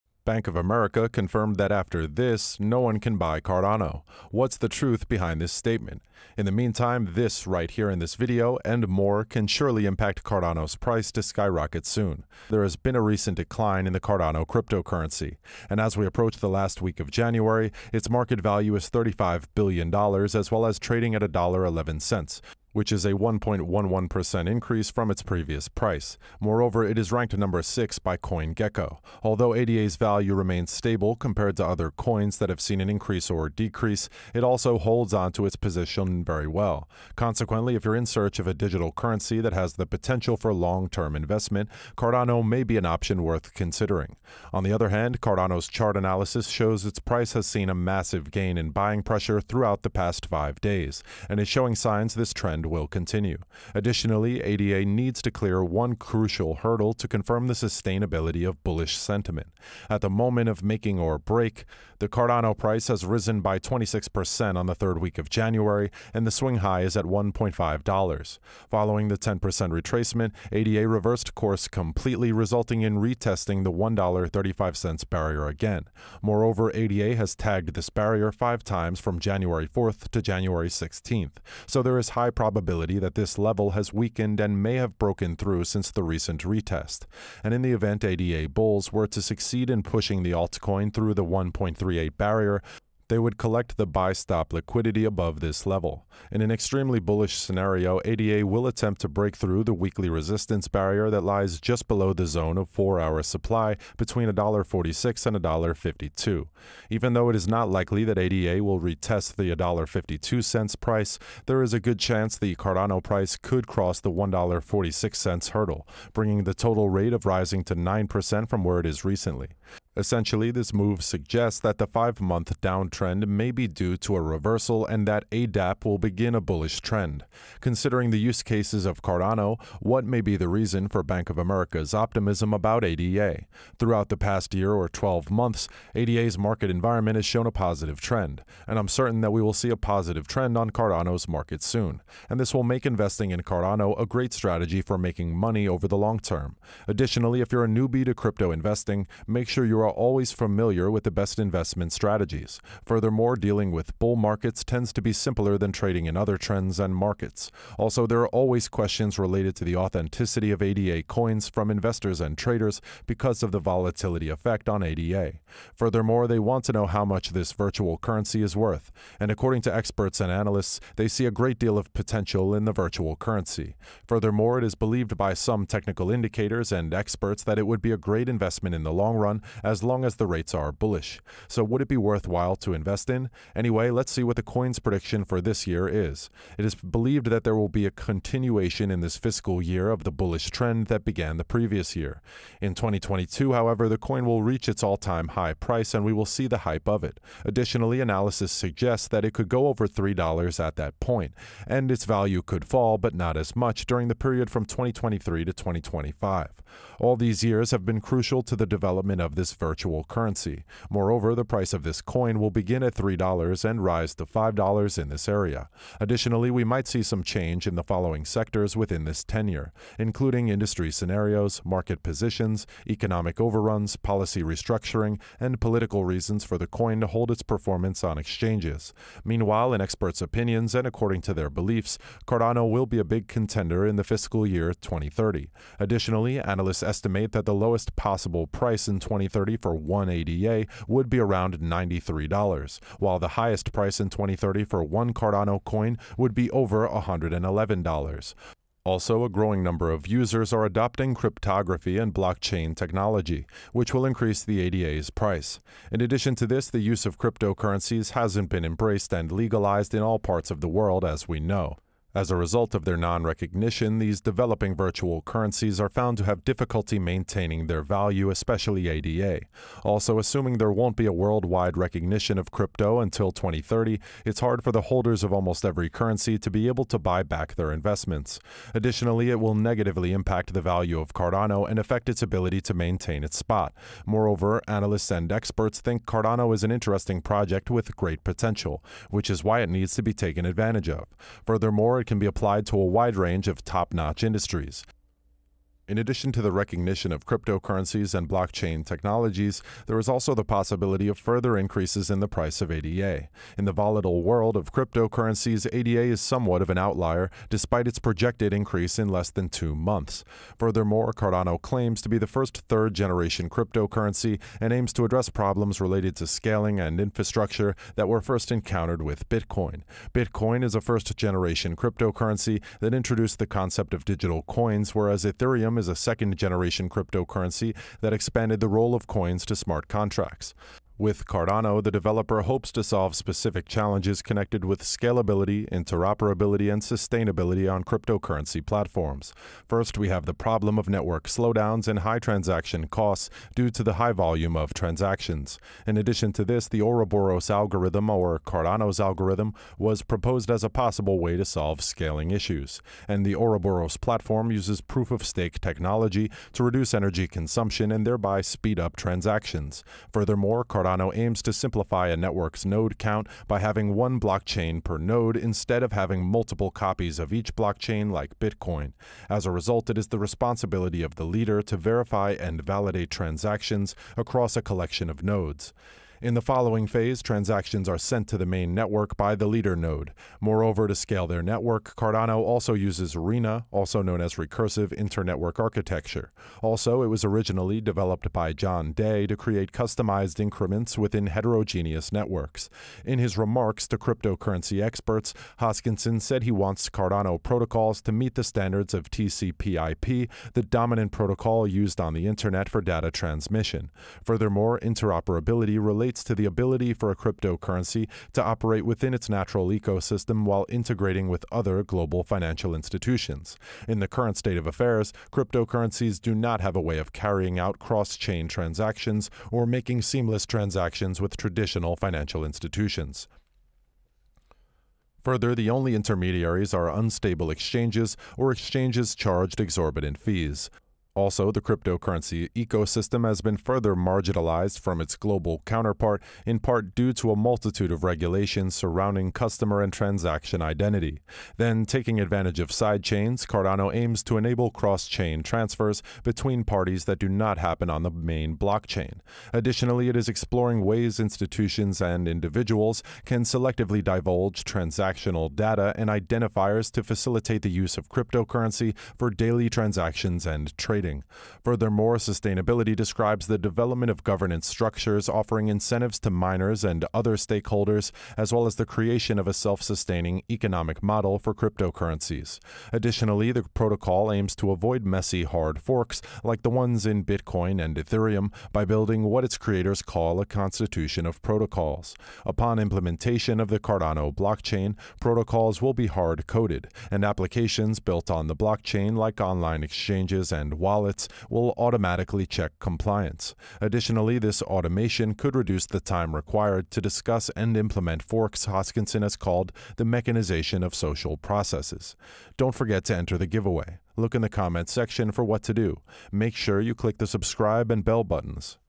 Bank Of America Confirmed: After THIS No One CAN Buy CARDANO!
In this video Cardano (ADA) is discussed. See the ADA News, what Cardano ADA is, and after what no one can buy Cardano (ADA) according to the Bank of America. Cardano is a public blockchain platform. It is open-source and decentralized, with consensus achieved using proof of stake. It can facilitate peer-to-peer transactions with its internal cryptocurrency ADA. Cardano was founded in 2015 by Ethereum co-founder Charles Hoskinson.

Different types of crypto are discussed and the latest crypto news is given to you!